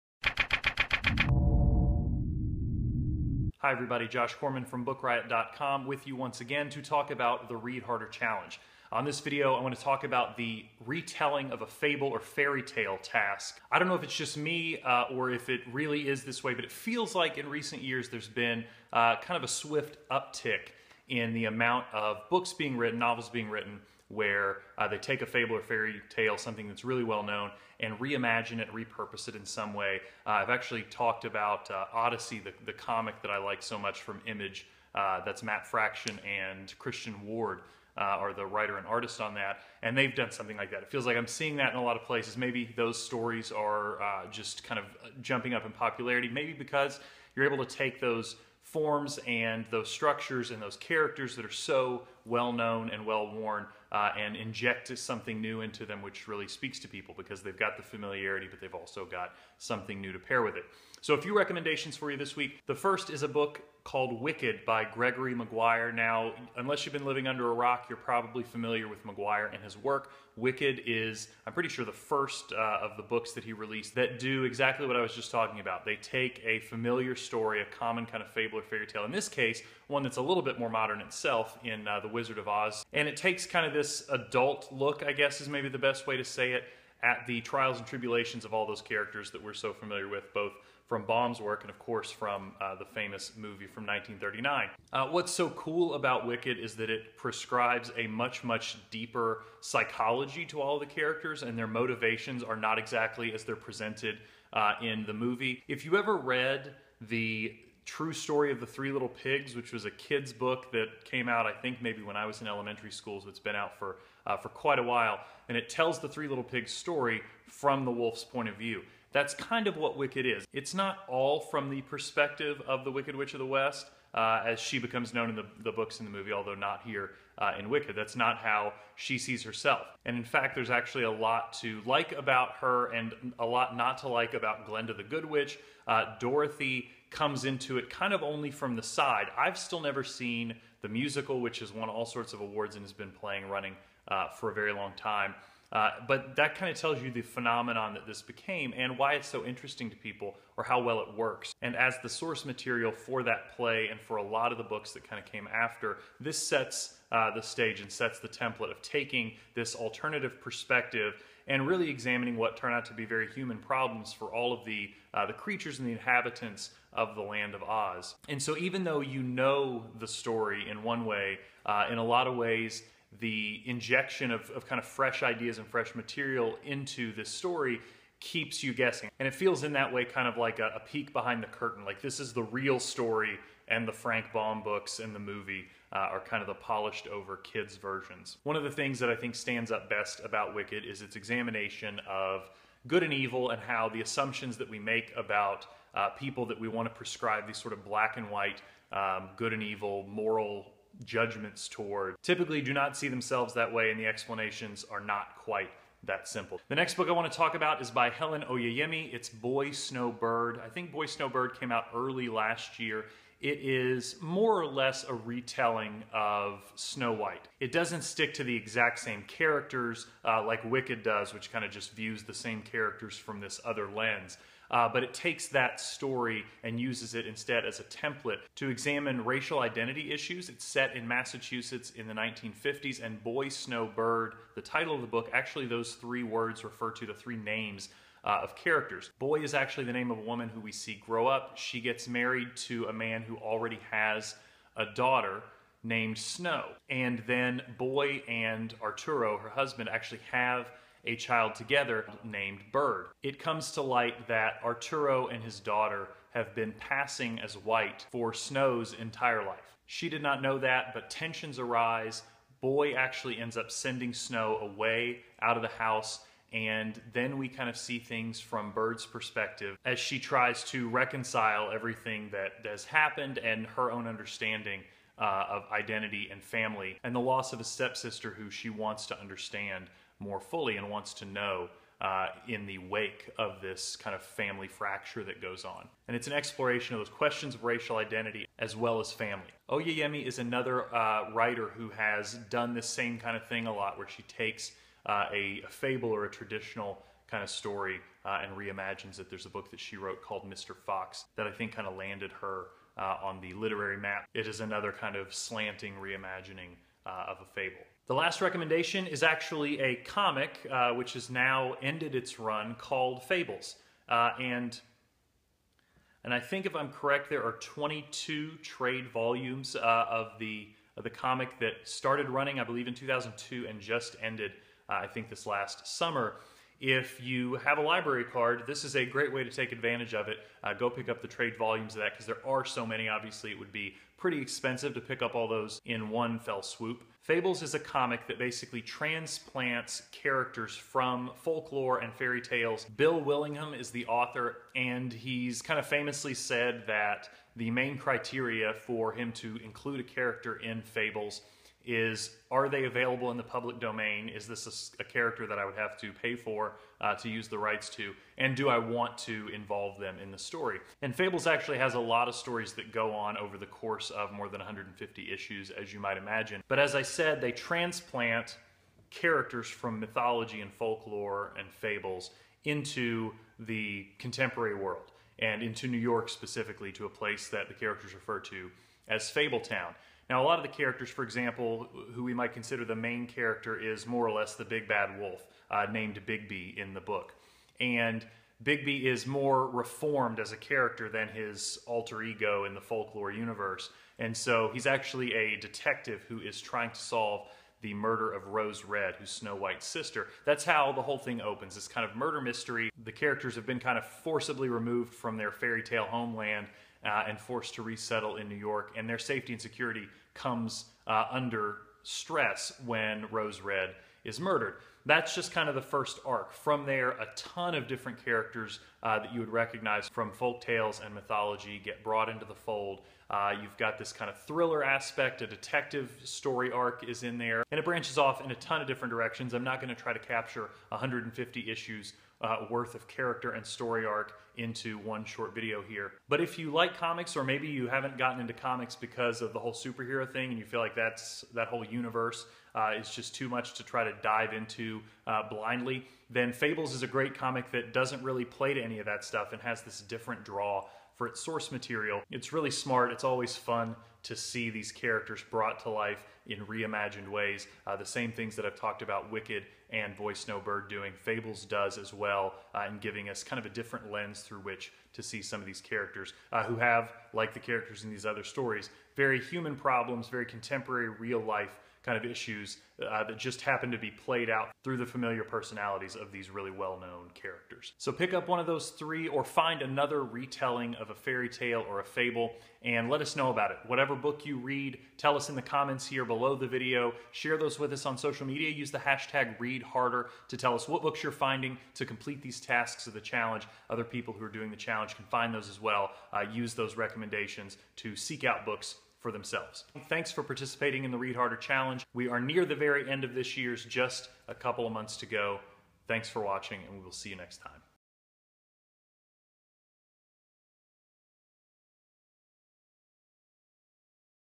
ReadHarder: A Retelling of a Fairytale or Fable
Back for more ReadHarder Challenge? In this video, Josh recommends a few retellings of fairytales, folklore and fables!